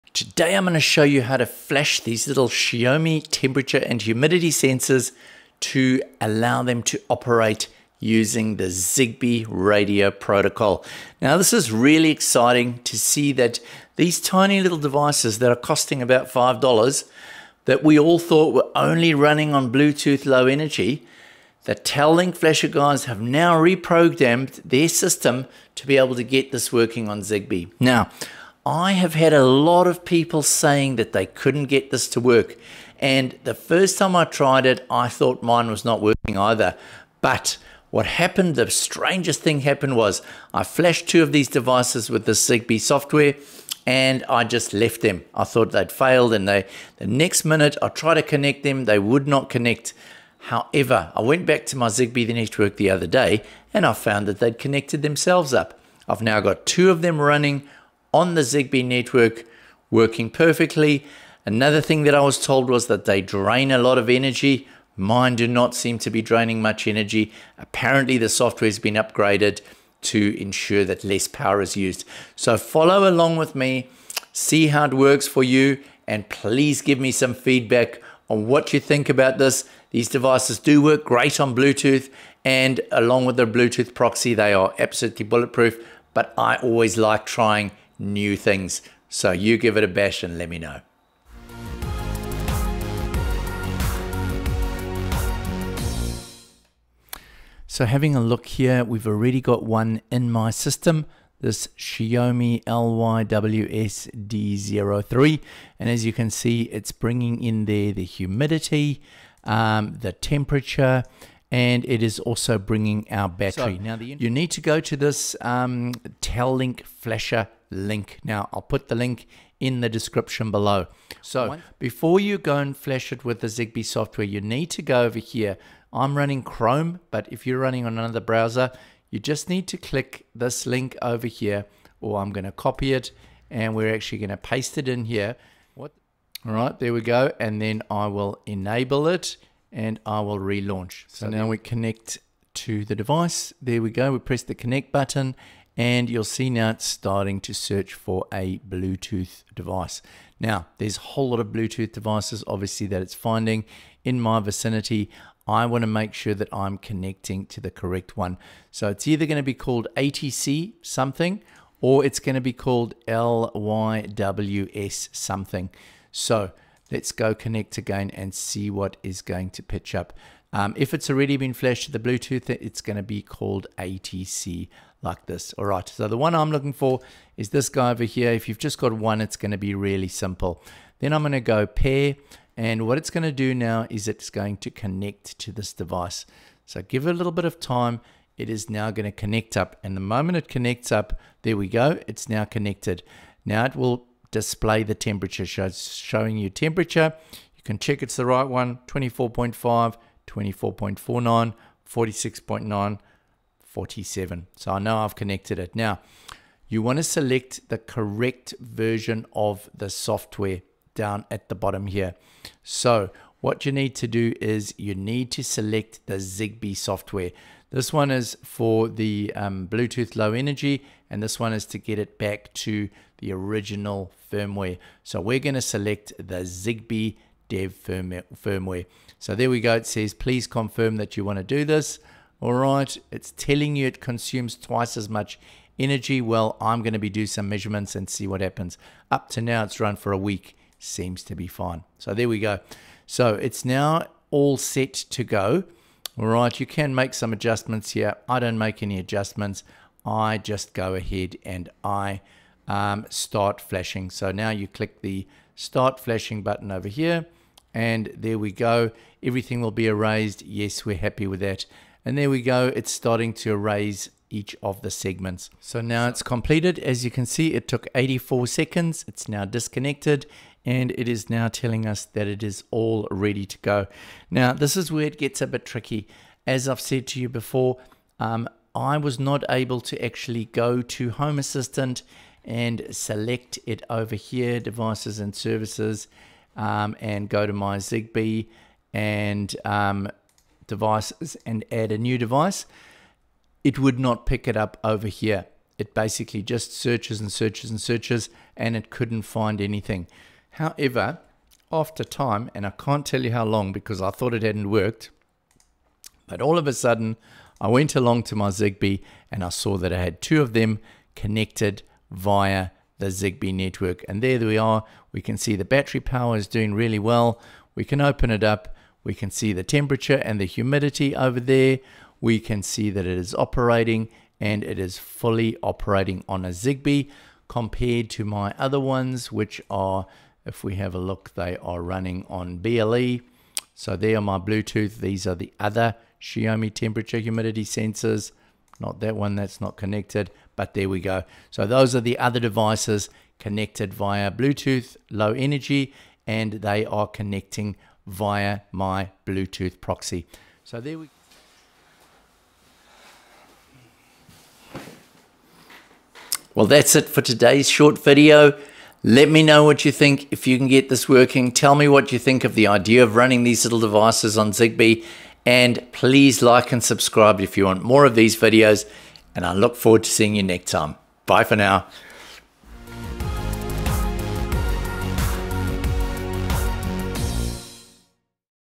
Cheapest $5 Xiaomi Temperature / Humidity Sensor now runs on Zigbee!
Xiaomi Temperature Humidity Sensor

My gear:
Rode Wireless Go Mic